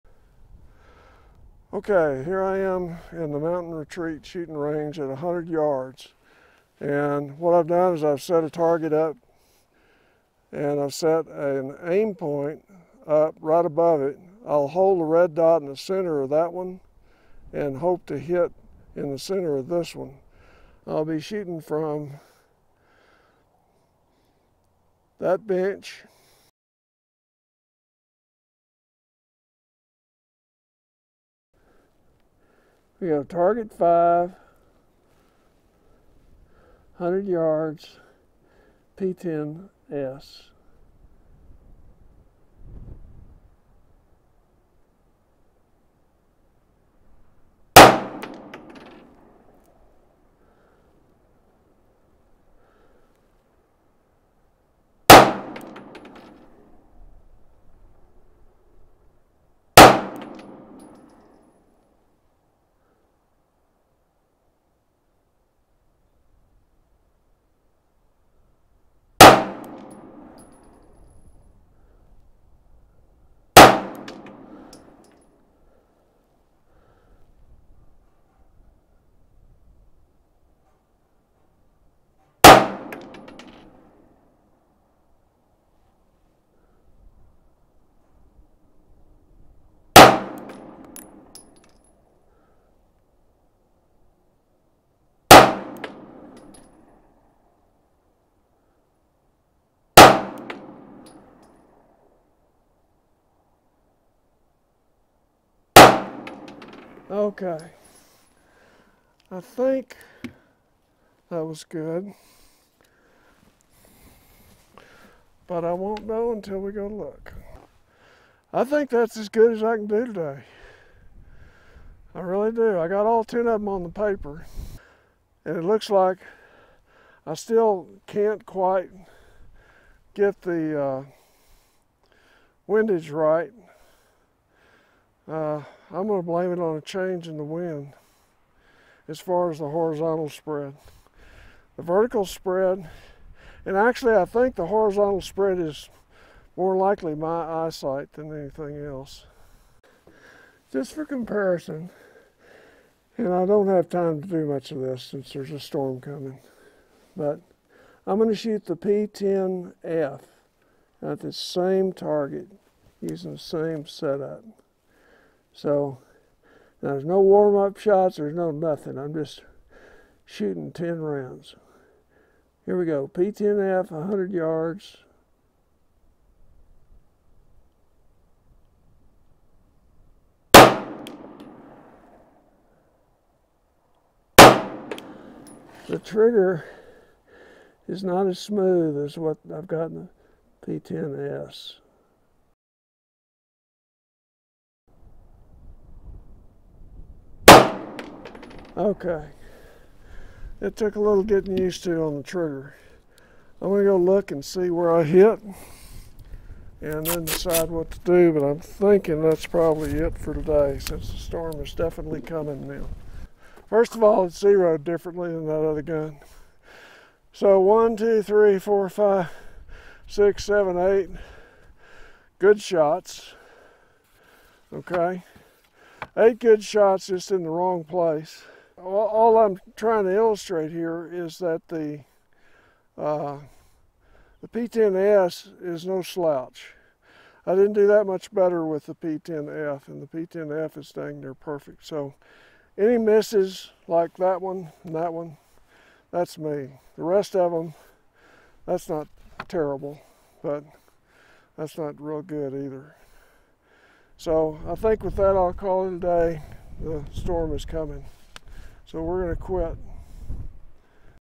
2019 11 21 P 10S and F at 100 yards
Second try at 100 yards with the P-10S.  Very windy and clouds coming and going made for some difficult conditions.  Results still OK, and I shot 10 rounds with the P-10F just for a standard check.